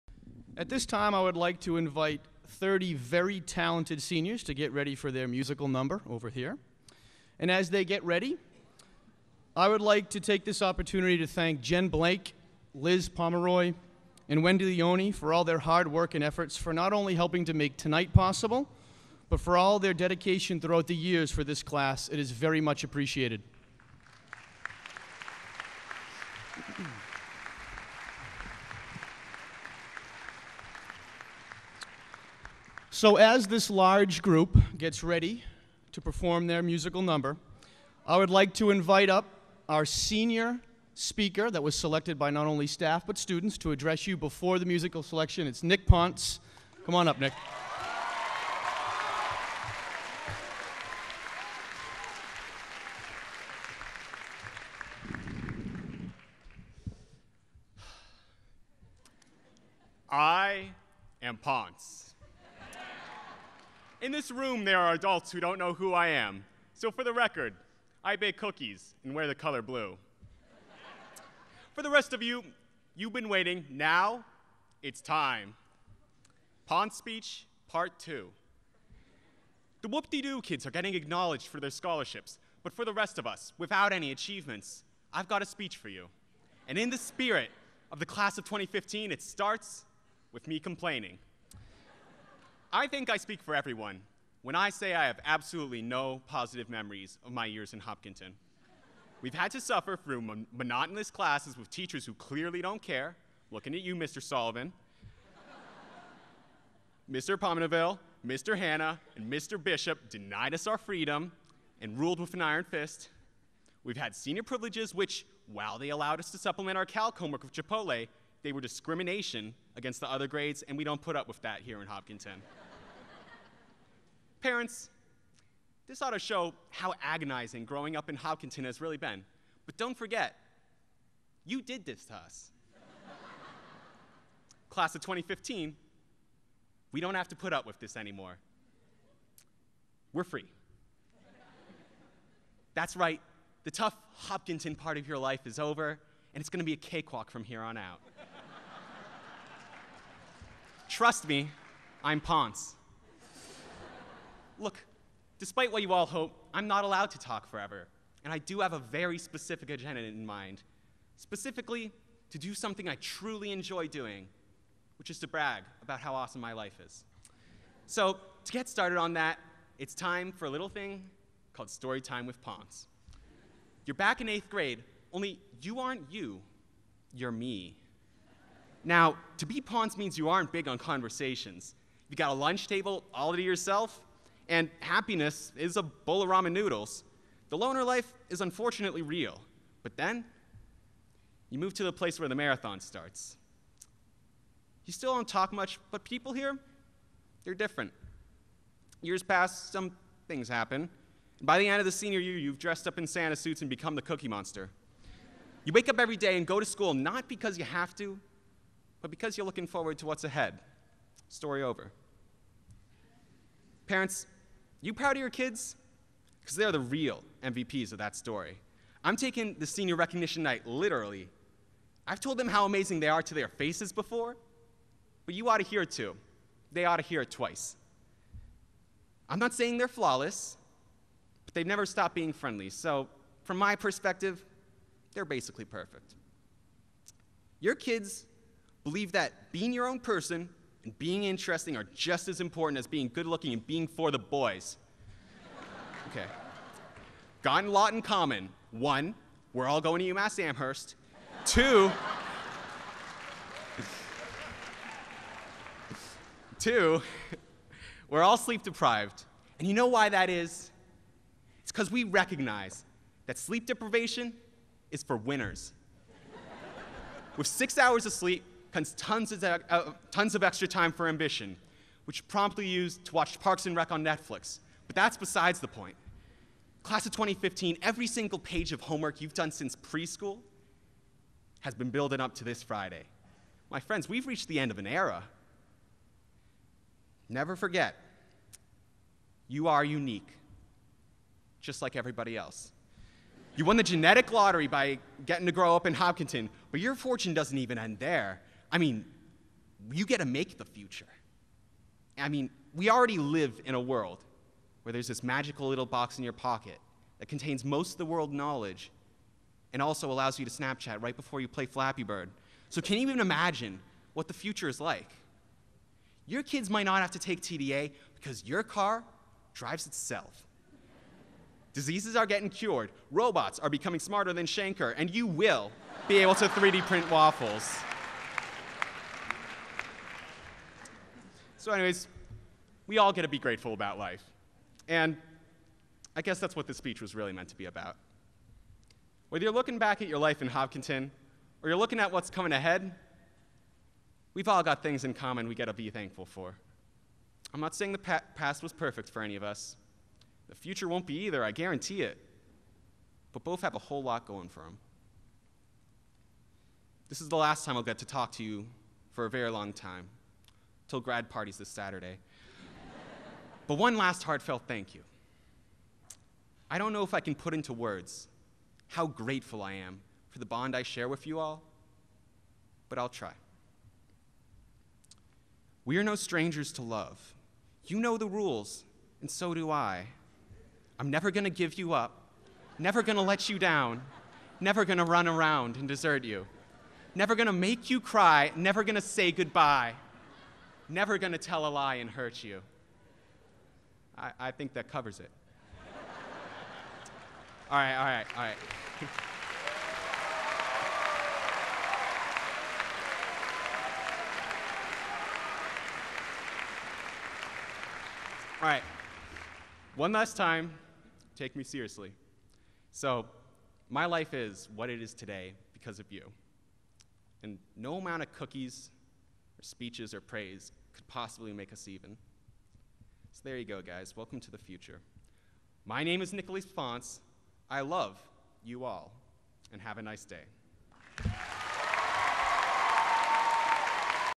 6-3-2015 Senior Recognition Night Nick Pontes
HHS Grad Nick Pontes shares some wisdom with his class.